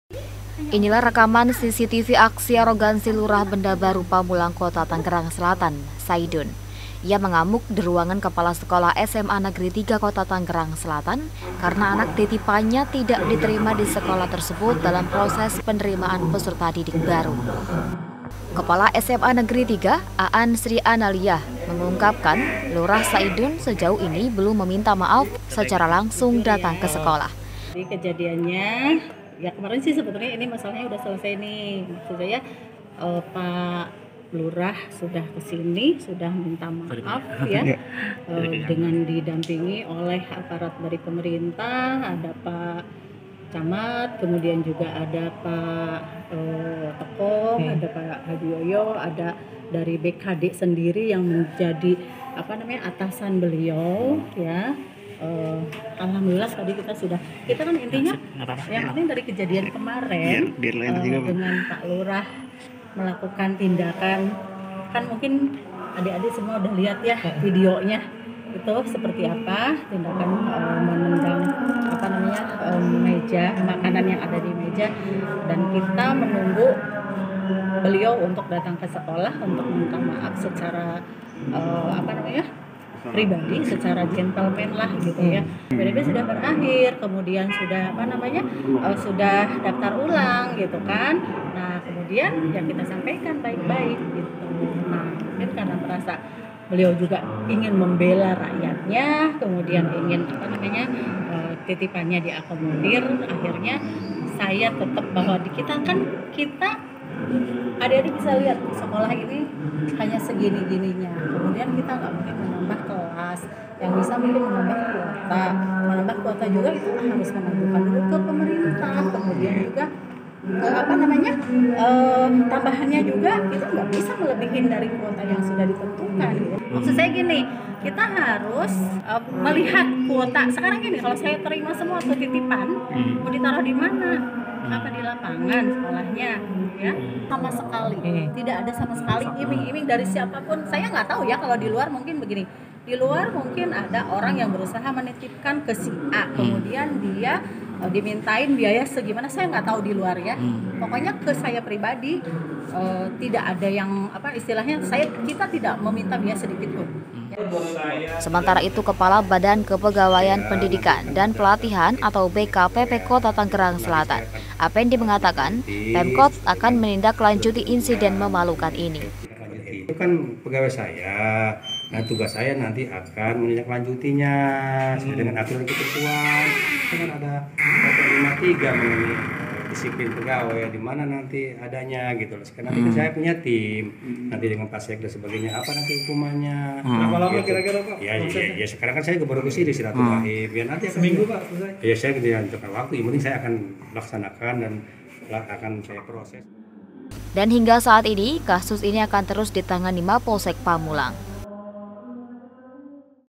Jatah Anak Titipan Tidak di Terima Disekolah Favorit - Berita Sultan
Pemirsa seorang lurah di kecamatan Pamulang Kota Tangerang Selatan, mengamuk di ruangan kepala Sekolah SMA Negri 3 di Kota Tangerang Selatan.

Dalam Rekaman CCTV yang menjadi viral di media socialitu dipicu lurah Benda Baru Saidun naik pitam karena anak titipanya tidak diterima disekolah tersebut dalam proses penerimaan peserta didik baru.